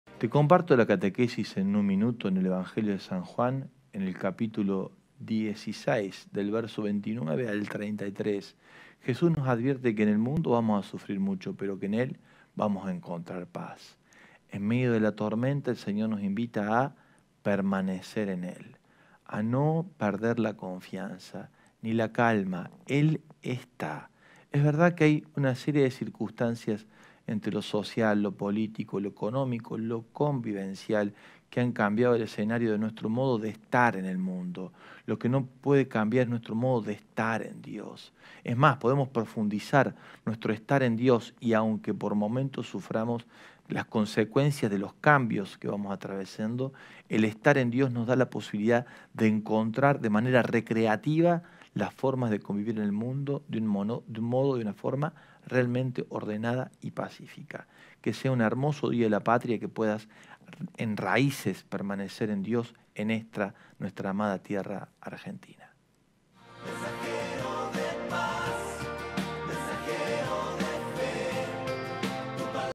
Tengan valor, yo he vencido al mundo - Catequesis en un minuto
En el Evangelio de hoy, Jn 16,29-33, Jesús nos advierte que en "el mundo tendrán que sufrir; pero tengan valor: yo he vencido al mundo". Nos promete que en Él encontraremos la paz. Jesús nos invita a permanecer en Él para no perder la calma.

En este tiempo, a causa de la pandemina, ha cambiado el modo de estar como humanidad en el mundo pero lo que no puede cambiar es nuestro modo de estar en Dios.

Que sea un hermoso día de la  Patria y que puedas, en raíces, permancer en Dios.